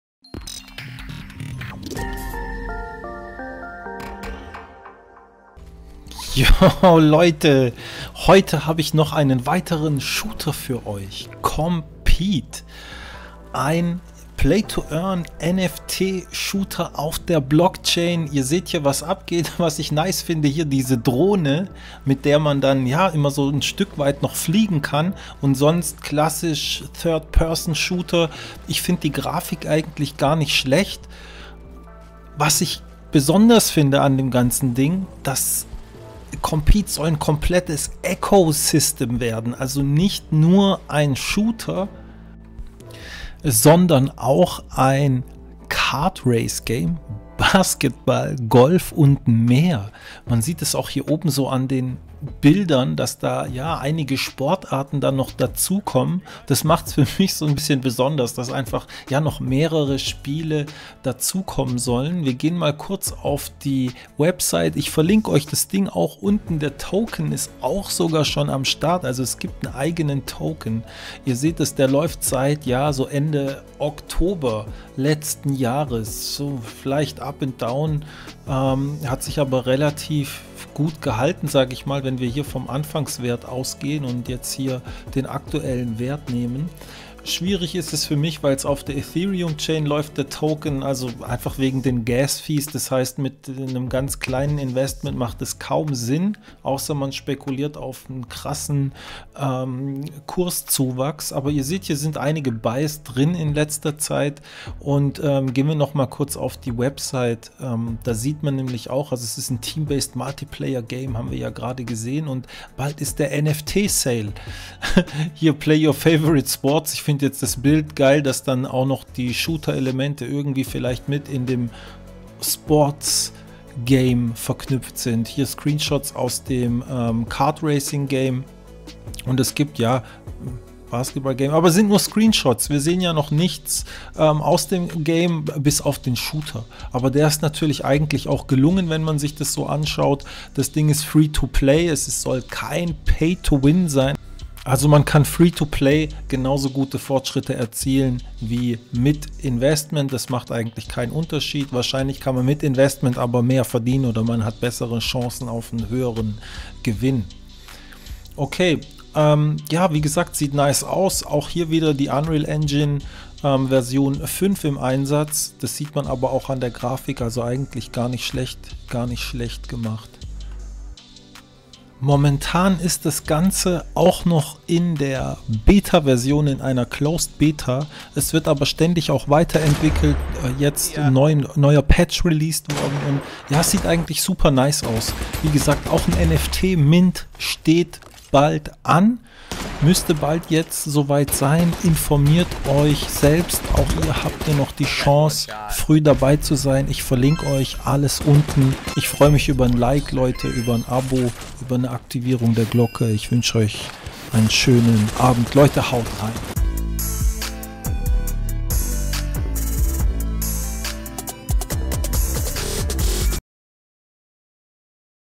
NFT SHOOTER 💥💥 FREE-TO-PLAY GAME 🎮🕹️ FORTNITE AUF DER BLOCKCHAIN?
FORTNITE AUF DER BLOCKCHAIN? TPS. Third Person Shooter Game. Free-to-play!

Free to play NFT Shooter Game , wie fortnite